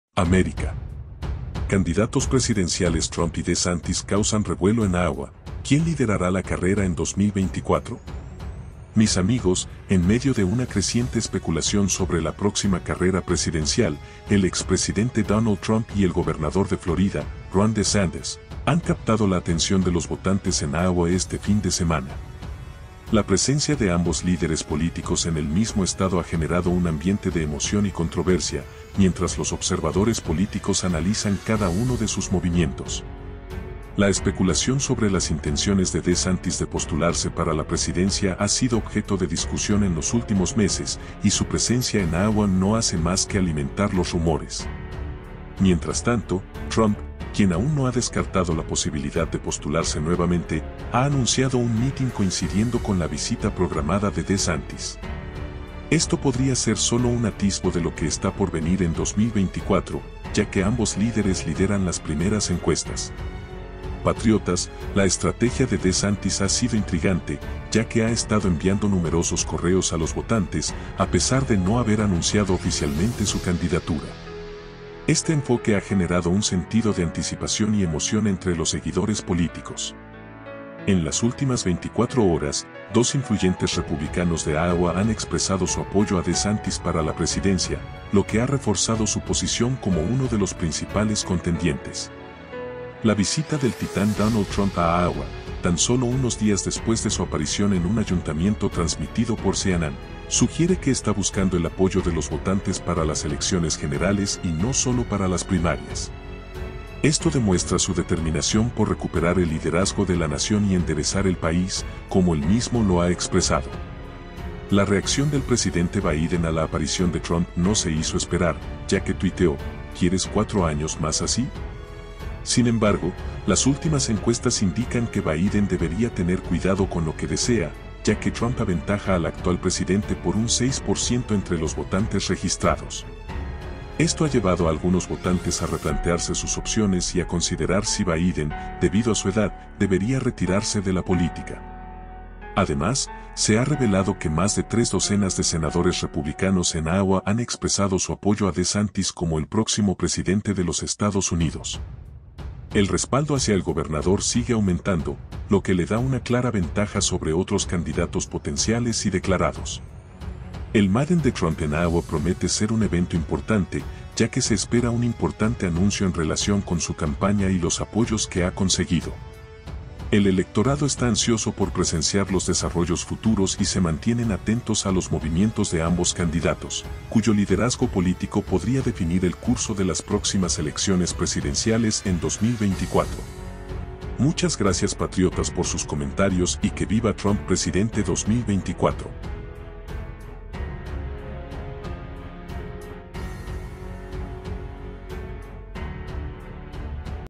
🚨🚨 EXTRA CARRERA PRESIDENCIAL TRUMP VS DESANTIS NOTICIAS DE ESTADOS UNIDOS 16 DE MAYO 2023
🚨🚨 EXTRA EXCLUSIVO  Trump Exige justicia Implacable Comey y demócratas NOTICIA DE ESTADOS UNIDOS 16 DE MAYO 2023